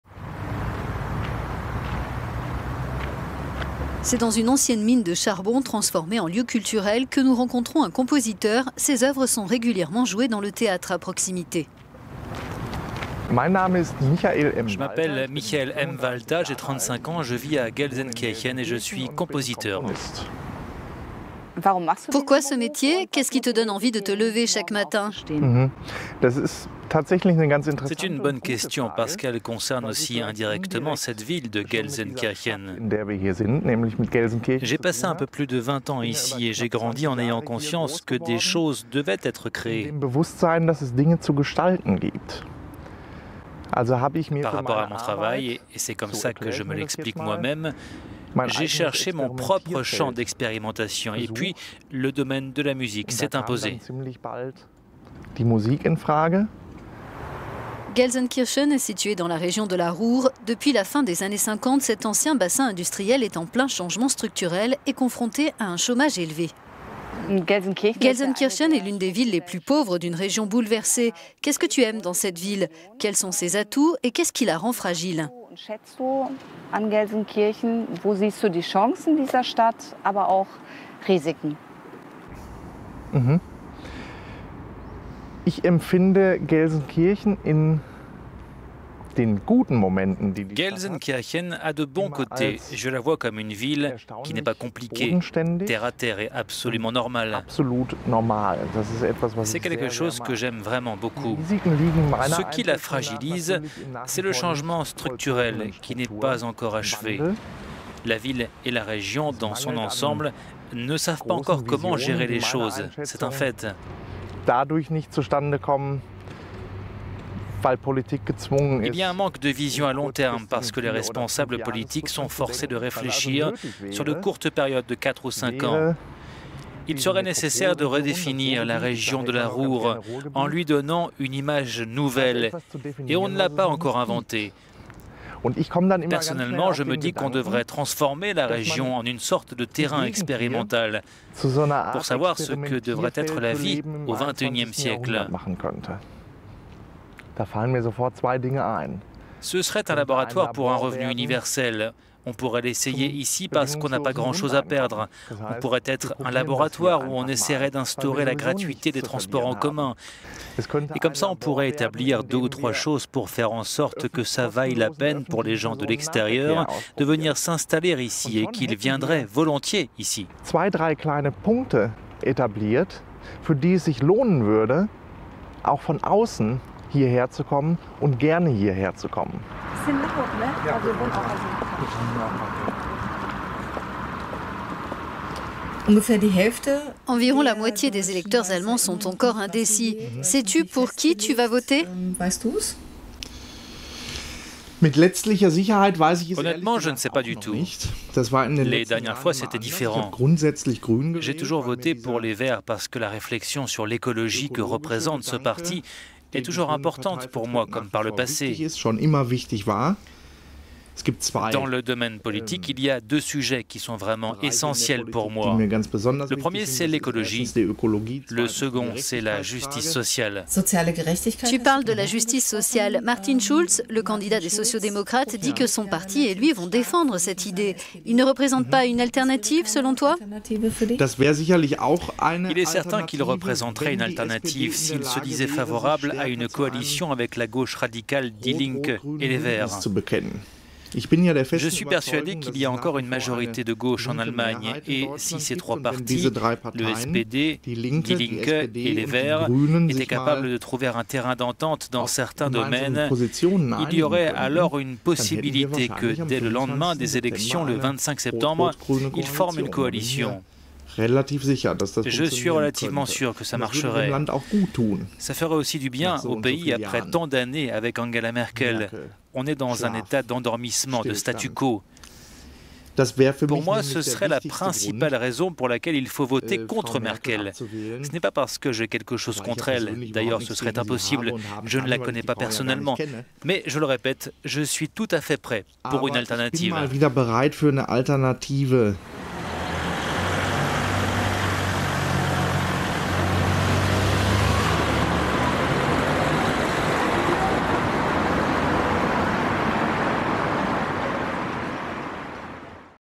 Élections allemandes 360° : la région de la Ruhr cherche le renouveau
Dans le cadre de notre série de vidéos à 360° sur les élections législatives allemandes prévues le 24 septembre, nous nous rendons à Gelsenkirchen dans la région de la Ruhr pour rencontrer un compositeur de musique classique qui vit sur place. Il nous dépeint une ville frappée par la pauvreté et un taux de chômage de 14% qui pourrait selon lui, avec de la volonté politique, devenir un "terrain expérimental pour savoir ce que devrait être la vie au XXIe siècle". Lui aussi attend une alternative à l'issue du prochain scrutin.